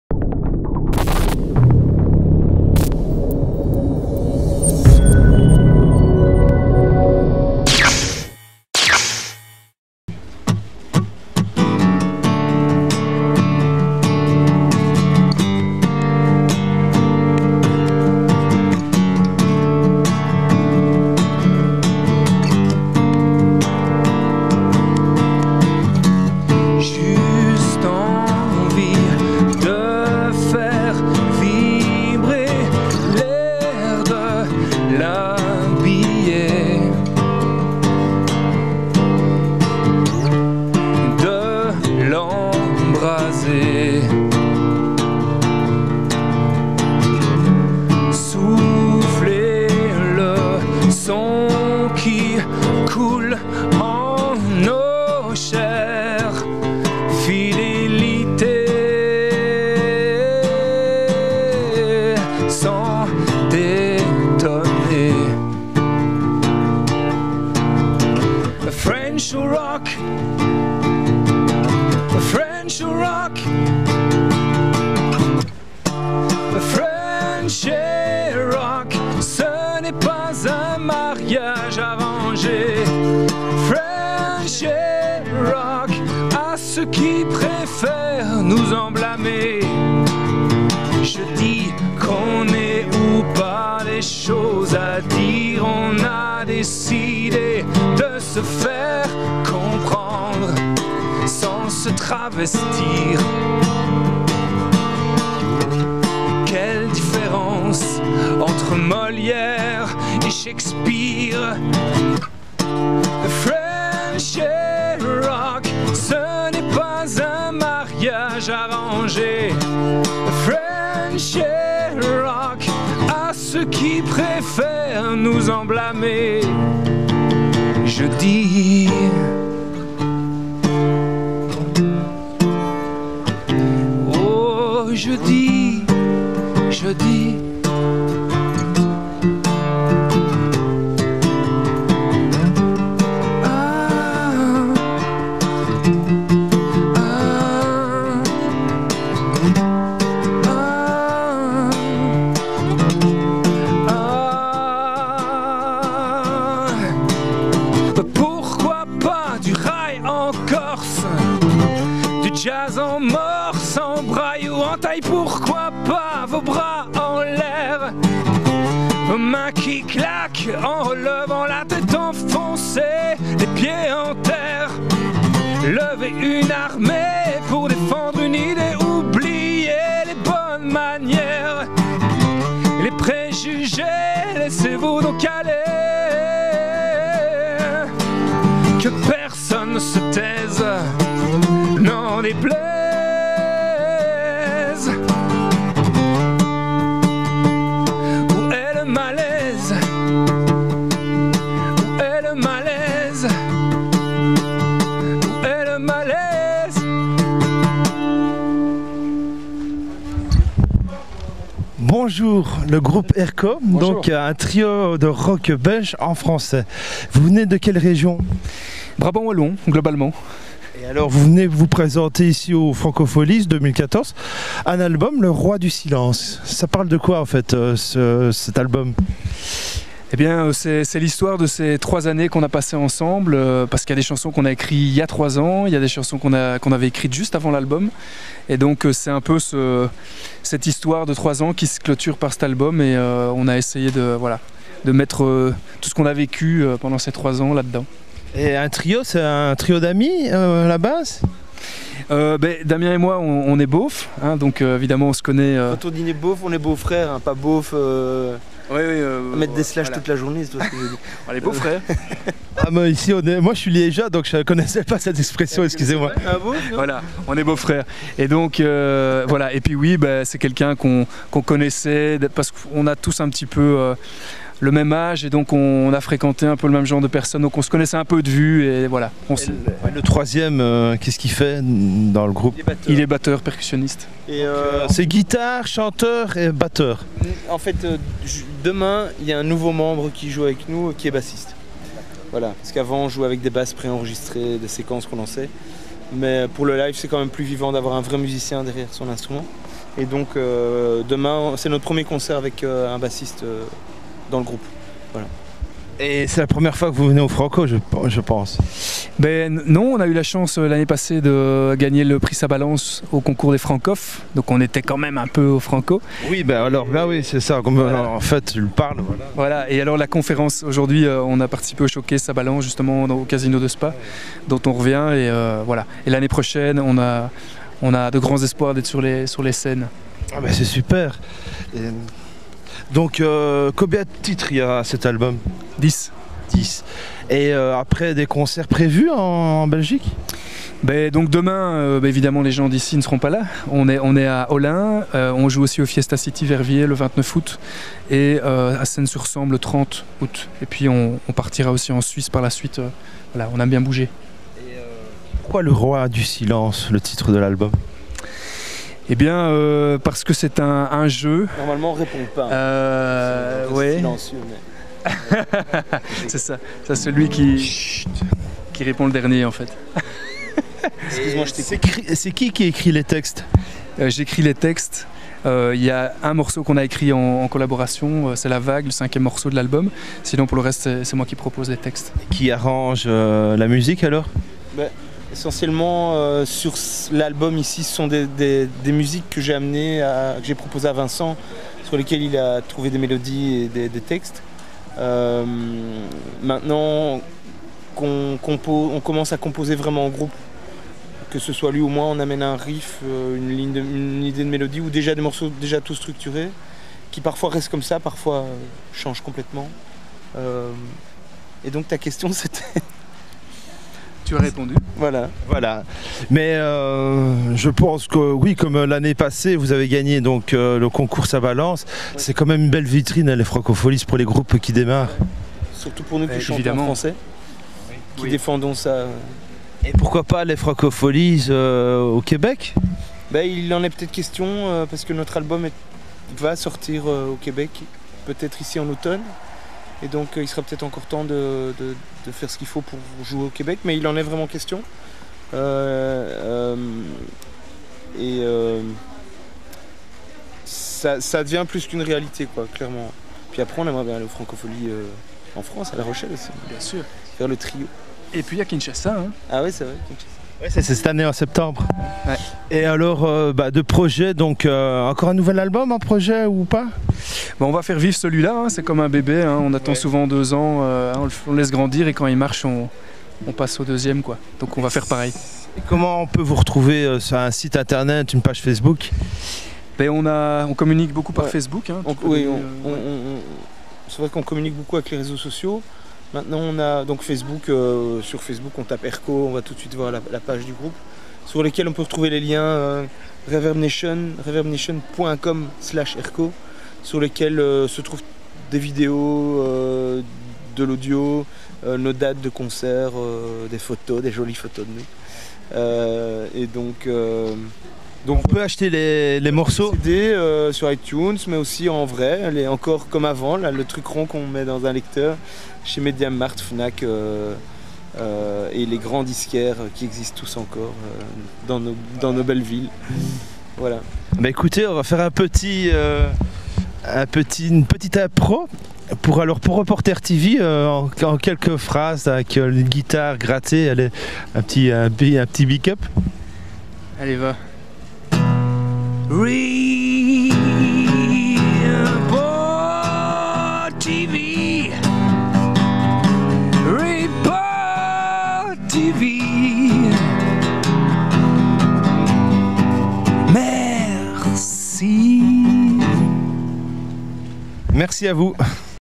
Airco aux Francofolies de Spa 2014
À propos
AIRCO du rock belge en français

Description
Damien Polfliet est un compositeur largement influencé par la musique anglo-saxonne. A la recherche de quelqu'un qui puisse lui souffler les paroles, Damien s'est tourné vers Vincent De Spiegeleer, un véritable complice, quant à lui plus inspiré dans sa langue maternelle. Ils ont donc construit ce projet en faisant le pari de faire sonner la musique rock anglo-saxonne en français. Accompagné de Pi... Afficher la suite